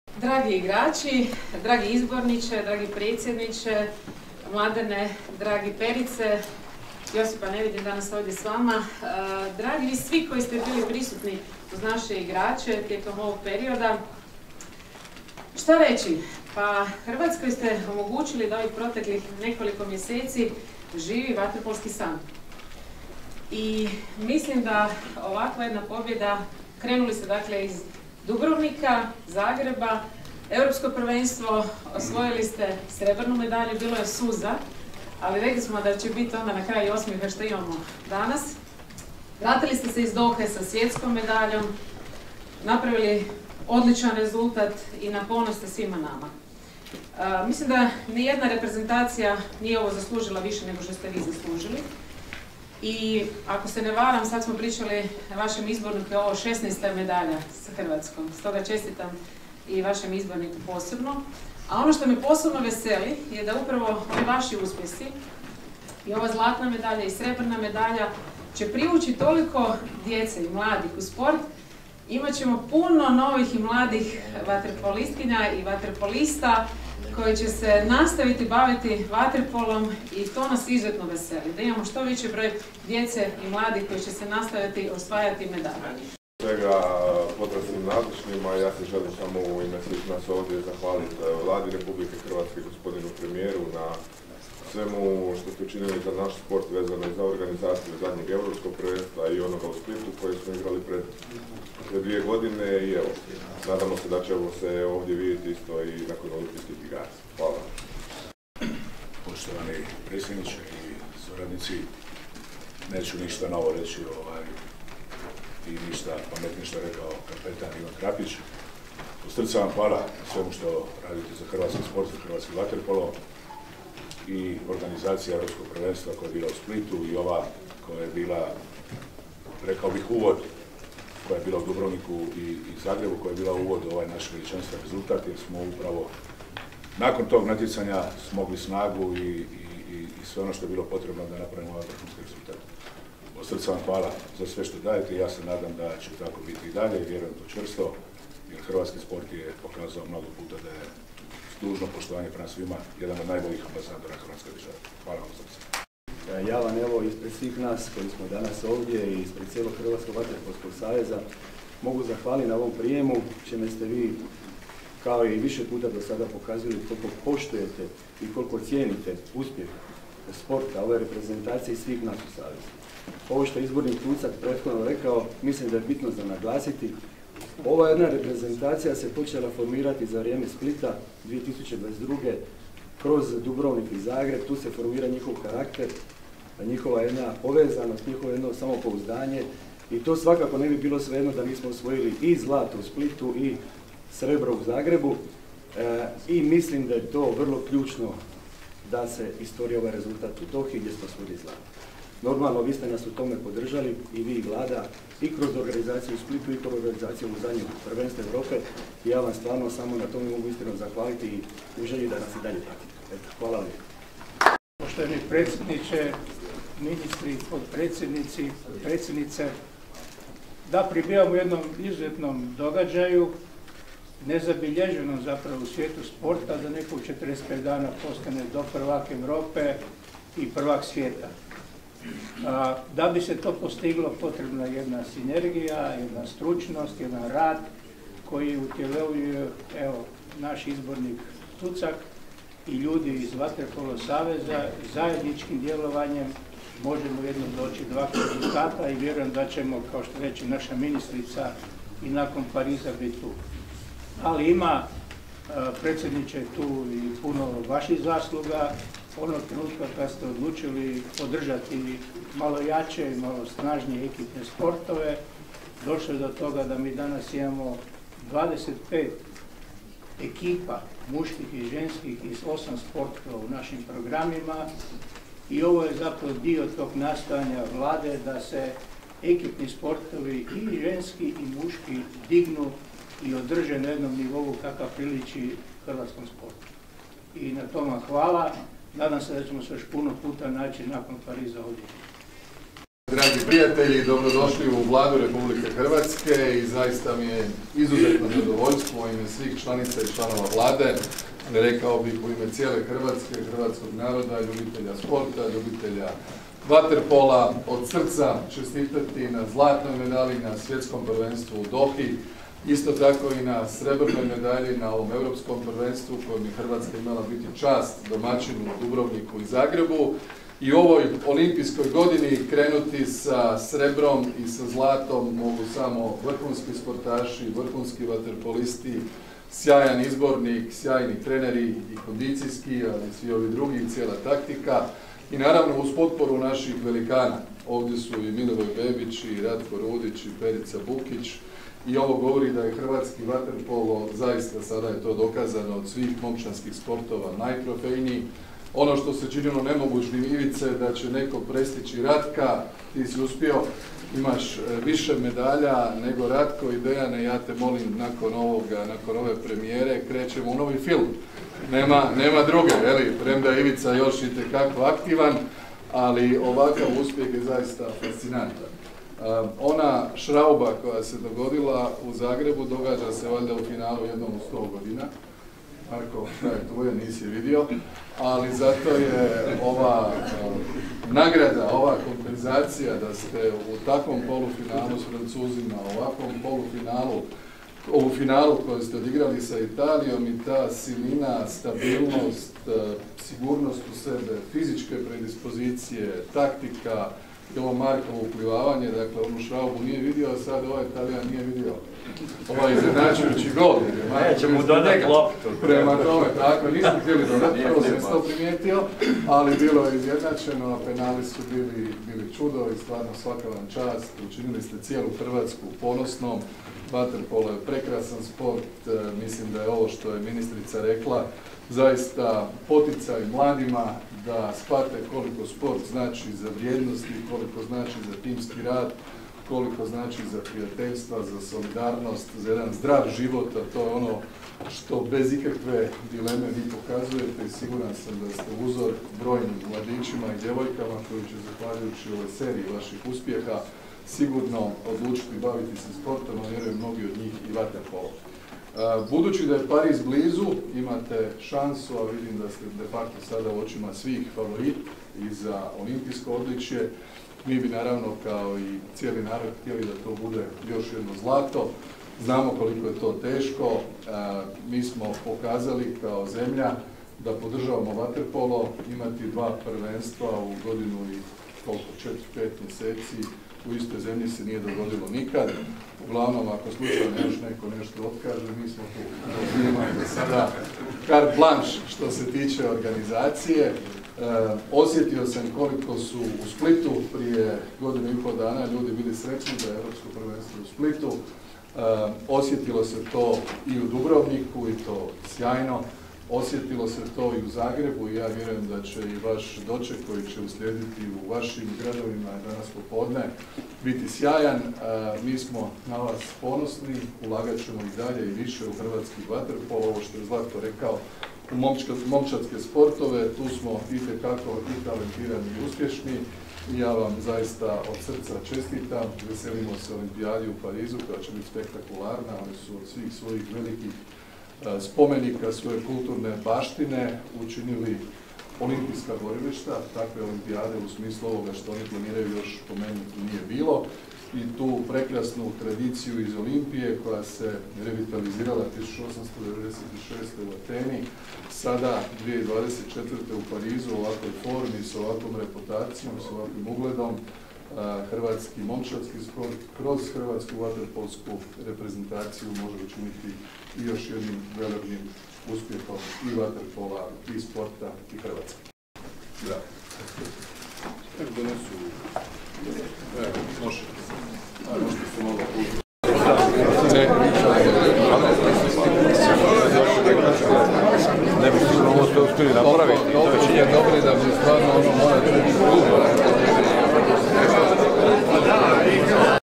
Andrej Plenković ugostio svjetske prvake u vaterpolu
Predsjednik Vlade Andrej Plenković primio je u povodu osvajanja zlatne medalje na Svjetskom prvenstvu u Dohi, hrvatske vaterpoliste i izaslanstvo Hrvatskog vaterpolskog saveza.